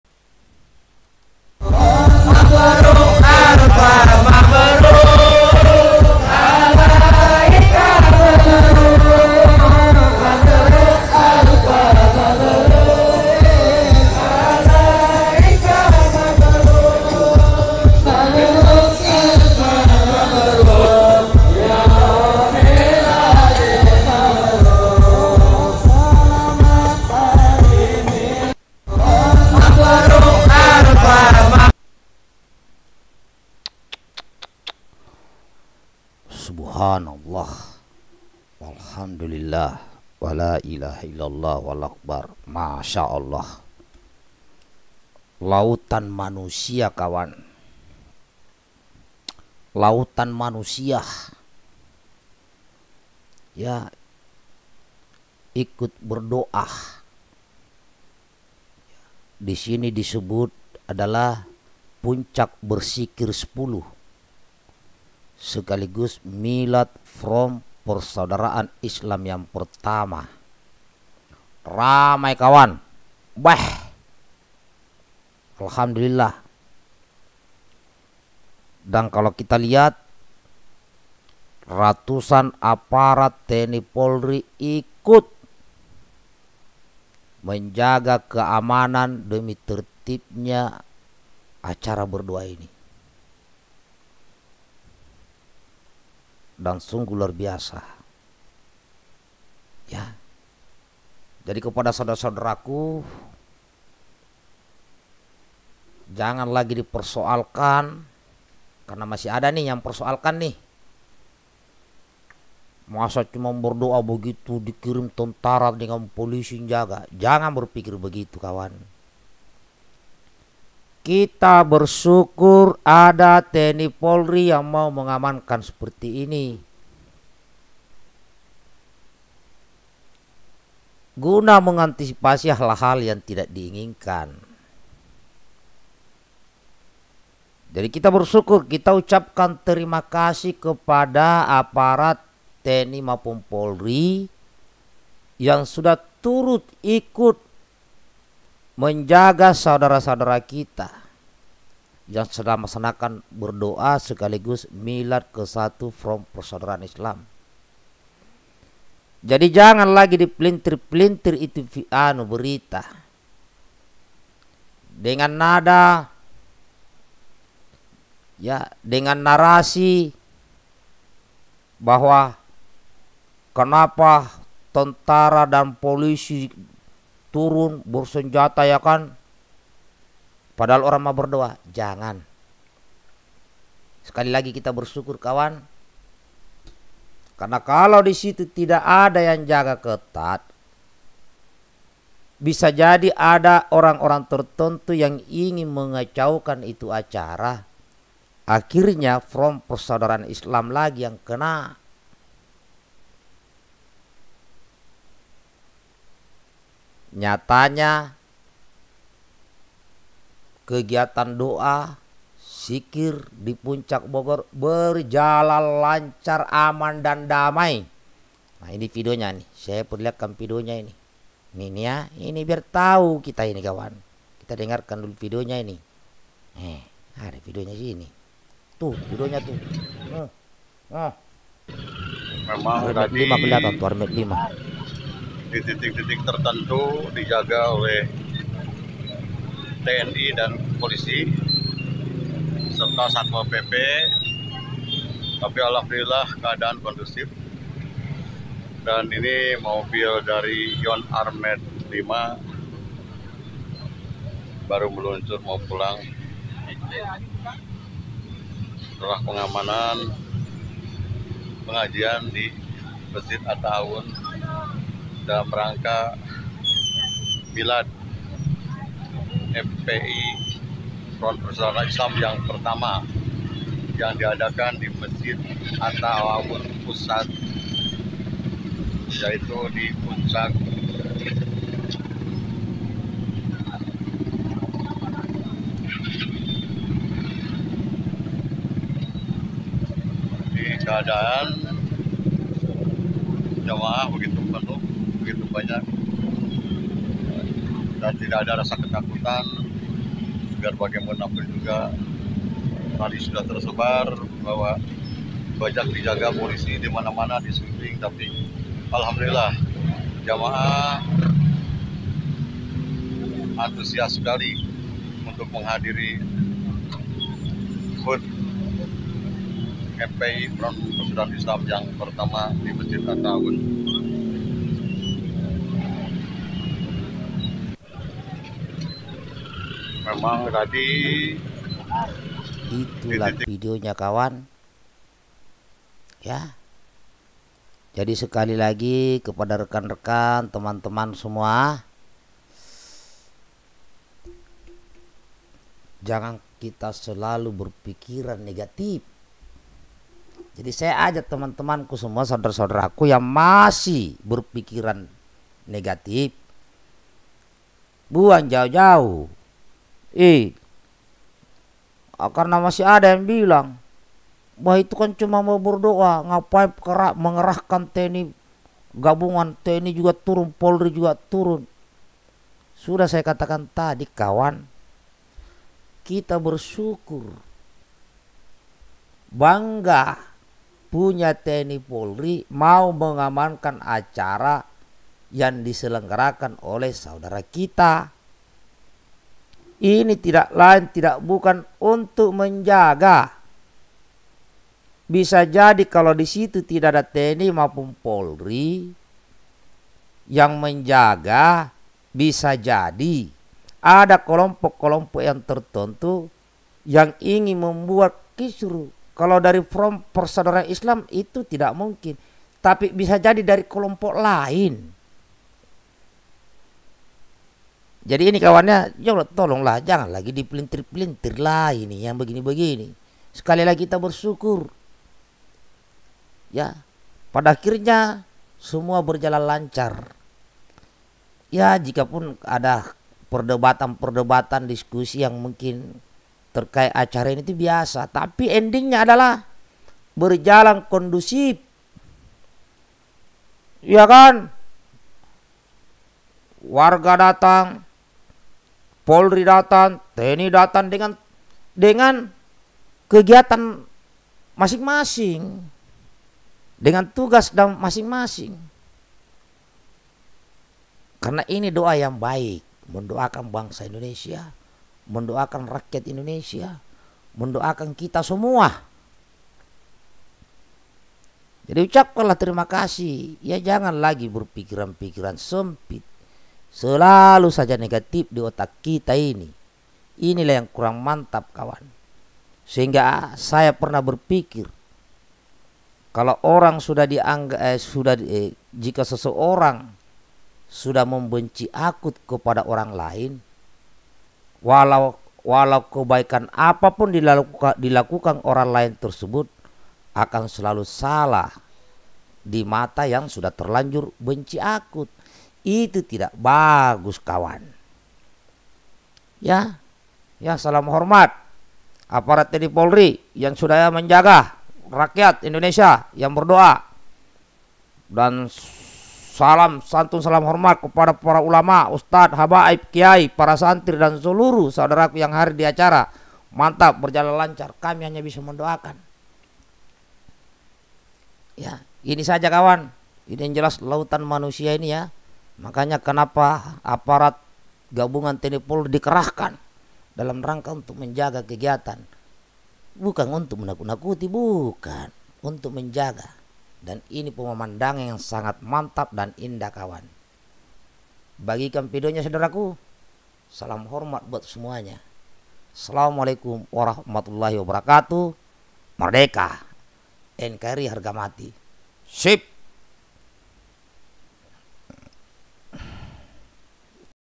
MILAD KE-1 F.P.ISLAM. Ratusan Aparat TNI-POLRI Dikerahkan, Lautan Manusia Berdo,a di Puncak Bogor.
Aamiin.

TTD